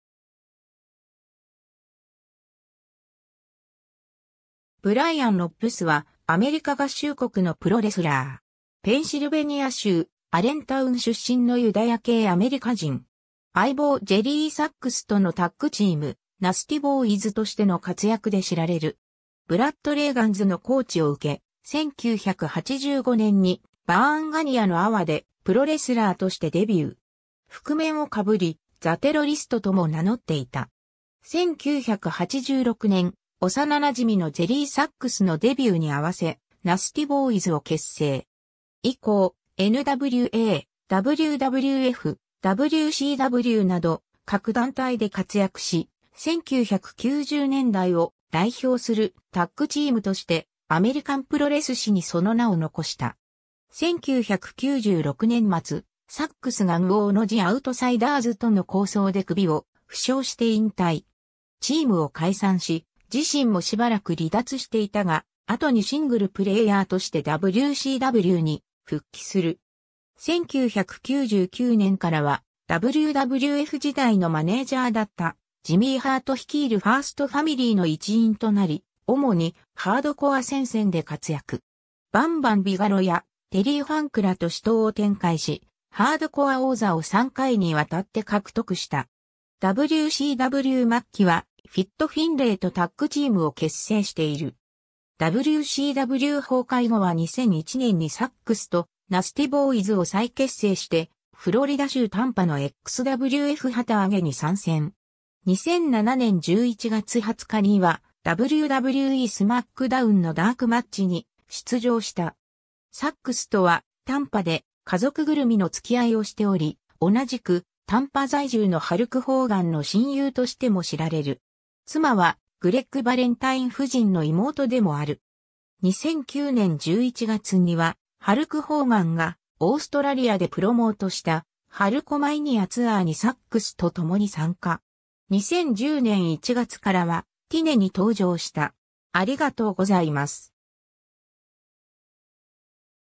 ブライアン・ノッブス
ブライアン・ノッブス（Brian Knobbs、本名：Brian Yandrisovitz、1964年5月6日 - ）は、アメリカ合衆国のプロレスラー。
ペンシルベニア州アレンタウン出身のユダヤ系アメリカ人。
相棒ジェリー・サッグスとのタッグチーム、ナスティ・ボーイズとしての活躍で知られる。
 ブラッド・レイガンズのコーチを受け、1985年にバーン・ガニアのAWAでプロレスラーとしてデビュー。
覆面を被り、ザ・テロリスト（The Terrorist）とも名乗っていた。
 1986年、幼なじみのジェリー・サッグスのデビューに合わせ、ナスティ・ボーイズ（The Nasty Boys）を結成。
以降、NWA、WWF、WCWなど各団体で活躍し、1990年代を代表するタッグチームとしてアメリカン・プロレス史にその名を残した。
 1996年末、サッグスがnWoのジ・アウトサイダーズ（ケビン・ナッシュ&スコット・ホール）との抗争で首を負傷して引退。
チームを解散し、自身もしばらく離脱していたが、後にシングルプレイヤーとしてWCWに復帰する。
1999年からはWWF時代のマネージャーだったジミー・ハート率いるファースト・ファミリー（The First Family）の一員となり、主にハードコア戦線で活躍。
バンバン・ビガロやテリー・ファンクらと死闘を展開し、ハードコア王座を3回に渡って獲得した。
WCW末期はフィット・フィンレイとタッグチームを結成している。
 WCW崩壊後は2001年にサッグスとナスティ・ボーイズを再結成してフロリダ州タンパのXWF（Xcitement Wrestling Federation）旗揚げに参戦。
2007年11月20日にはWWEスマックダウンのダーク・マッチに出場した。
 サッグスとはタンパで家族ぐるみの付き合いをしており、同じくタンパ在住のハルク・ホーガンの親友としても知られる。
妻はグレッグ・バレンタイン夫人の妹でもある。
 2009年11月にはハルク・ホーガンがオーストラリアでプロモートした "Hulkamania Tour" にサッグスと共に参加。
2010年1月からはTNAに登場した。